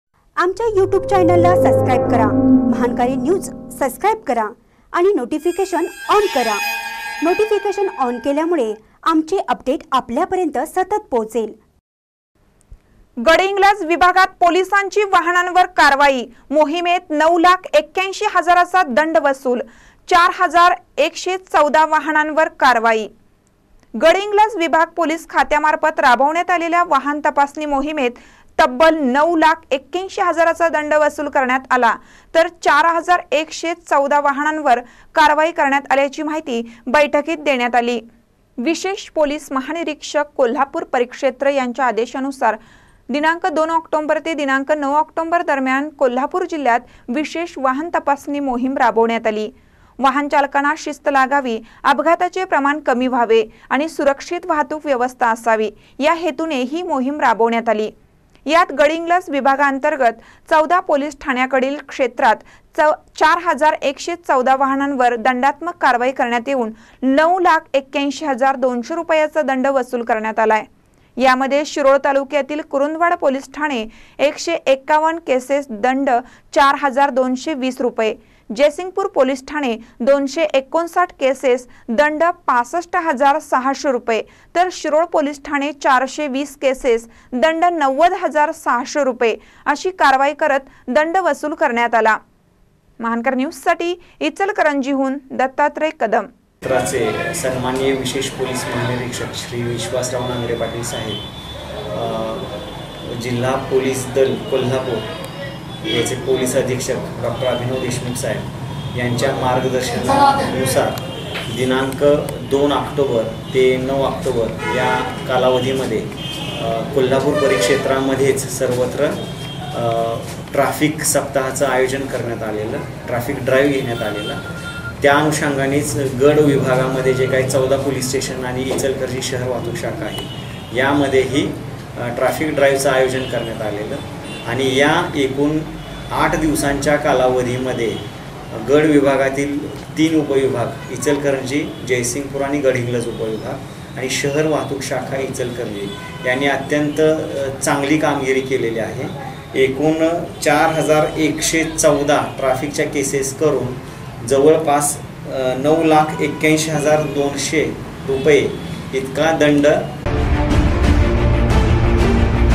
गडहिंग्लज विभागात पोलीसांची वाहनांवर कारवाई : मोहीमेत ९ लाख ८१ हजाराचा
गडहिंग्लज विभागात पोलीसांची वाहनांवर कारवाई : मोहीमेत ९ लाख ८१ हजाराचा दंड वसूल,  ४ हजार ११४ वाहनांवर कारवाई